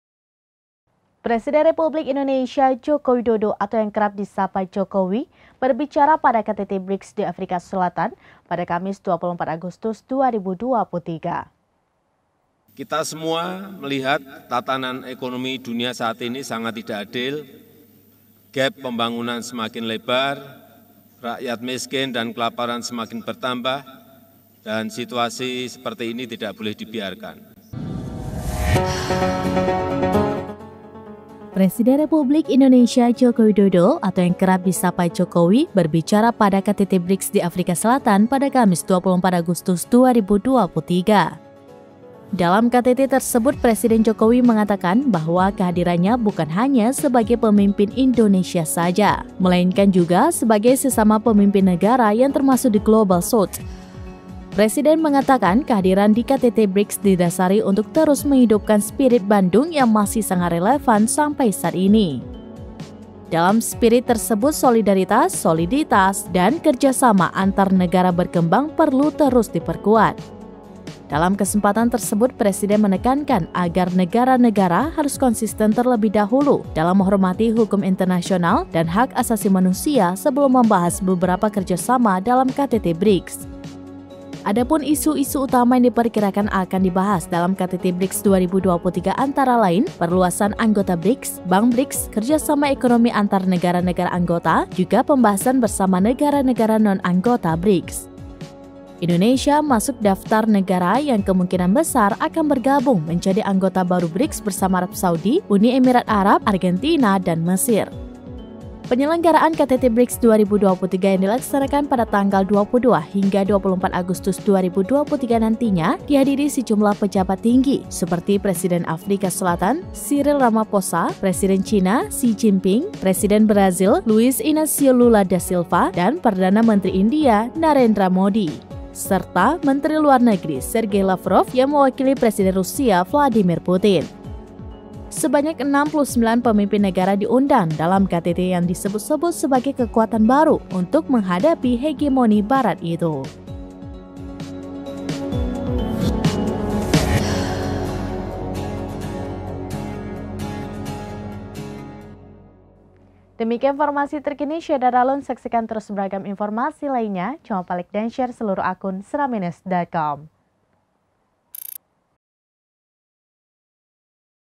Presiden Jokowi Bicara Win-Win Formula saat Hadir di KTT BRICS
SERAMBINEWS.COM - Presiden Republik Indonesia Joko Widodo atau yang kerap disapa Jokowi berbicara pada KTT BRICS di Afrika Selatan, Kamis (24/8/2023).

Dalam KTT tersebut, Presiden Jokowi mengatakan bahwa kehadirannya bukan hanya sebagai pemimpin Indonesia saja.

Melainkan juga sebagai sesama pemimpin negara yang termasuk the global south.

Presiden mengatakan kehadiran di KTT BRICS didasari untuk terus menghidupkan "spirit Bandung" yang masih sangat relevan sampai saat ini.

Dalam spirit tersebut solidaritas, soliditas dan kerjasama antar negara berkembang perlu terus diperkuat.

Dalam kesempatan tersebut, Presiden menekankan agar negara negara harus konsisten terlebih dahulu dalam menghormati hukum internasional dan hak asasi manusia sebelum membahas berbagai kerjasama dalam KTT BRICS.

Adapun isu-isu utama yang diperkirakan bakal dibahas dalam KTT BRICS 2023 antara lain, perluasan anggota BRICS, Bank BRICS, kerja sama ekonomi antar negara-negara anggota, juga pembahasan bersama negara-negara non-anggota BRICS.

Indonesia masuk daftar negara yang kemungkinan besar akan bergabung menjadi anggota baru BRICS bersama Arab Saudi, Uni Emirat Arab, Argentina, dan Mesir.

Penyelenggaraan KTT BRICS 2023 yang dilaksanakan pada tanggal 22-24 Agustus 2023 nantinya dihadiri sejumlah pejabat tinggi seperti Presiden Afrika Selatan Cyril Ramaphosa, Presiden China Xi Jinping, Presiden Brazil Luiz Inacio Lula da Silva, dan Perdana Menteri India Narendra Modi.

Serta Menteri Luar Negeri Sergei Lavrov yang mewakilkan Presiden Rusia Vladimir Putin.

Sebanyak 69 pemimpin negara diundang dalam KTT yang disebut-sebut sebagai kekuatan baru untuk menghadapi hegemoni Barat itu.(*)

VO : Syita
EV : Aziz